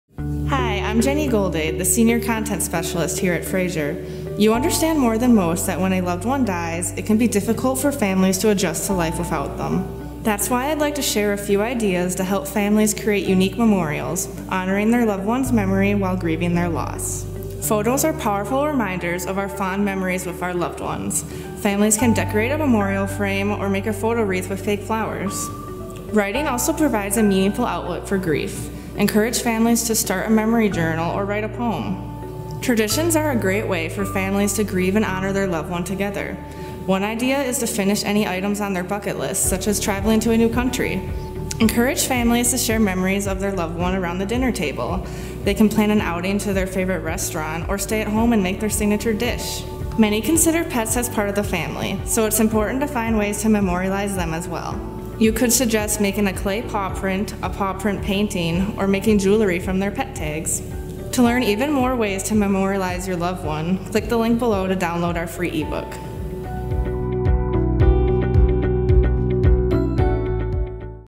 Creative Ways Families Can Memorialize Their Loved One
When a loved one passes away, it’s difficult for families to adjust to life without them. One way to keep their presence alive is by creating meaningful memorials. This helps families remember their loved one while they cope with their grief.

One way to memorialize a loved one is to use photographs of them in different ways. One idea is to decorate a memorial frame with pictures that show fond memories of the deceased. Or, they could create a photo wreath that incorporates the pictures with fake flowers.

Writing is another great way to memorialize a loved one. Families can create a memory journal or even write a poem about their loved one. Writing is a cathartic way to express one’s grief.

Creating new traditions that honor the deceased are another great way to honor them. For instance, families could finish any remaining items on their loved one’s bucket list. They could also plan a family outing to their loved one’s favorite restaurant or stay at home and cook their favorite recipe.

Let’s also keep in mind that many consider their pets a part of the family. To memorialize a late pet, families can make a clay pay print, a paw print painting, or make jewelry from their pet’s tag.